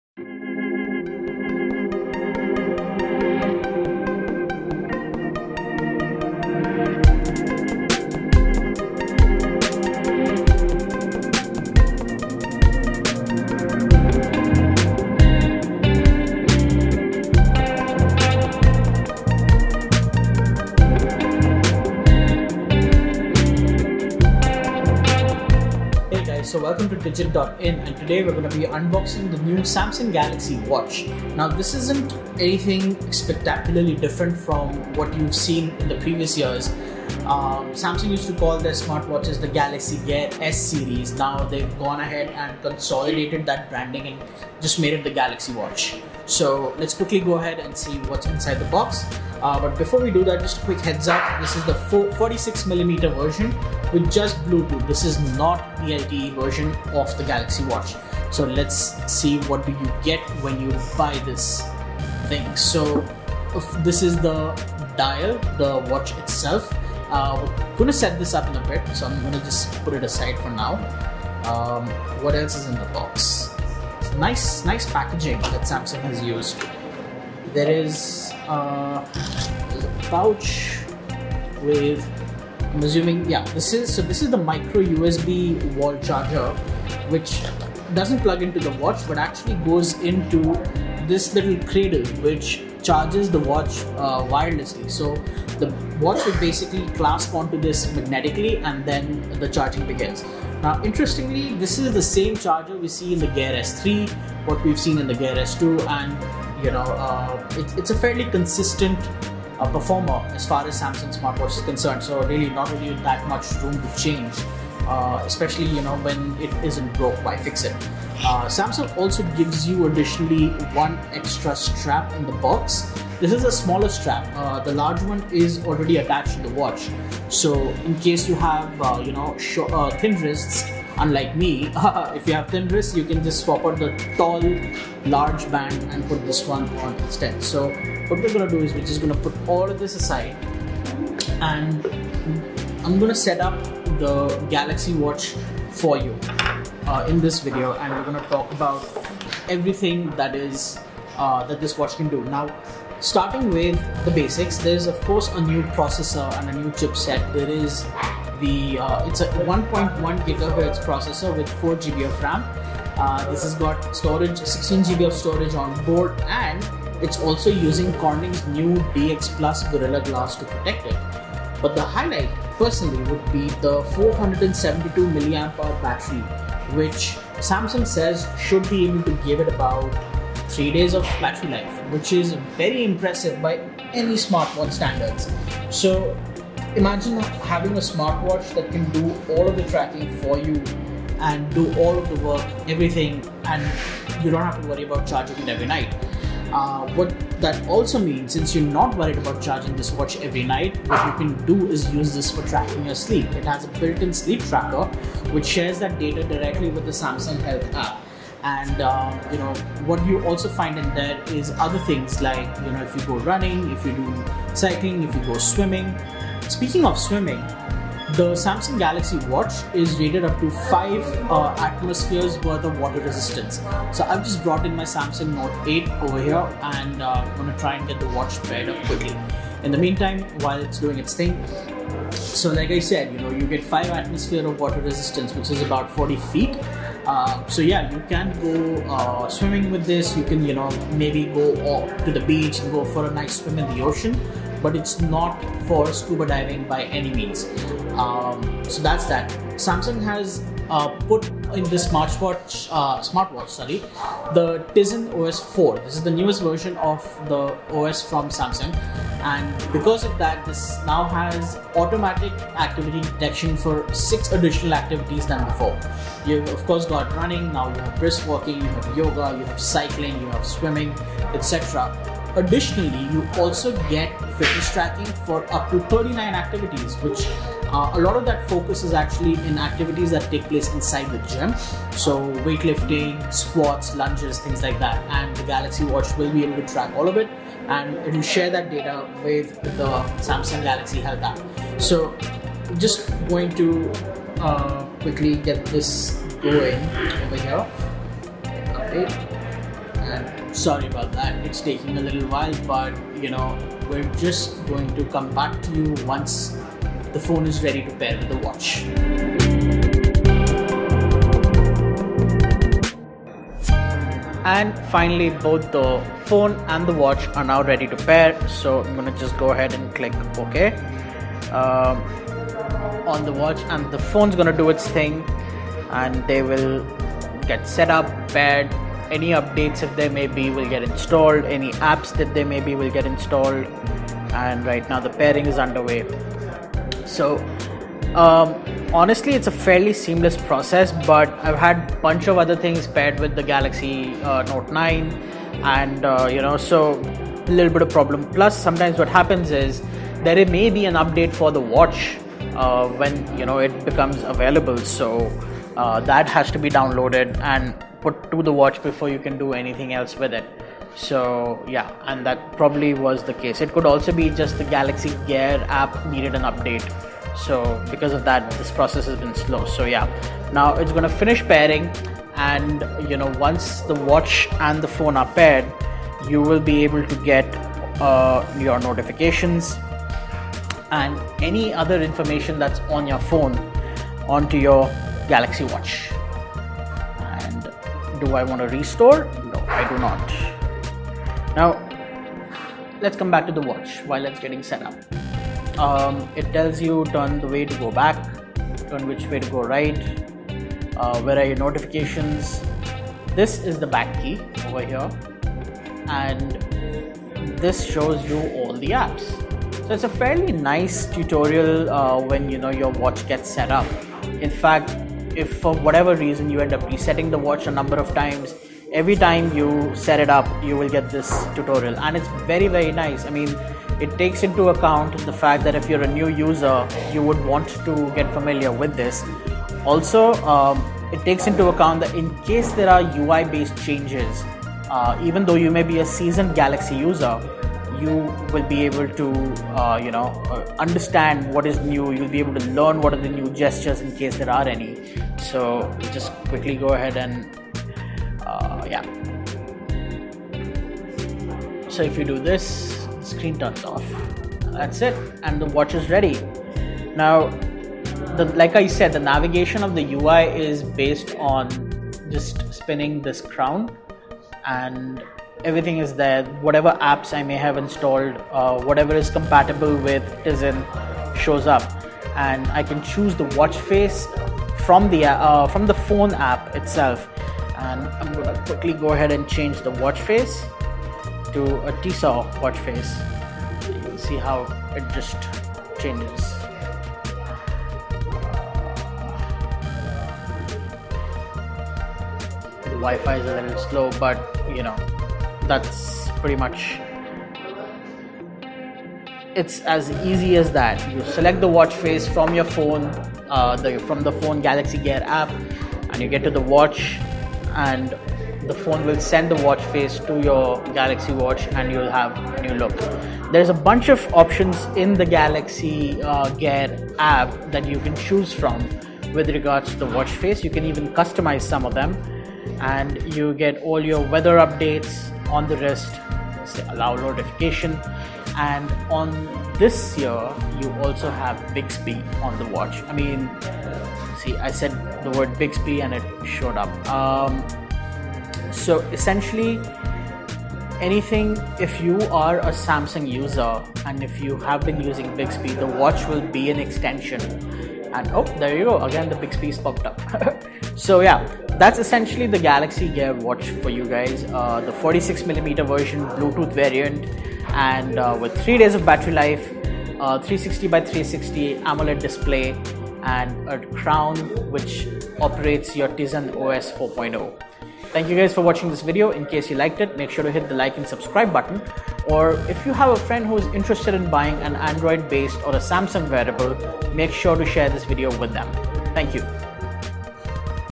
Samsung Galaxy Watch - Unboxing, Setup & First Impressions | Digit.in
The Samsung Galaxy Watch is a smartwatch that lasts 4 days on regular use, tracks all your activities with great accuracy and delivers smooth operation. It is a worthy upgrade for anyone who wants an Android-compatible smartwatch with great battery life and the looks of an actual wristwatch.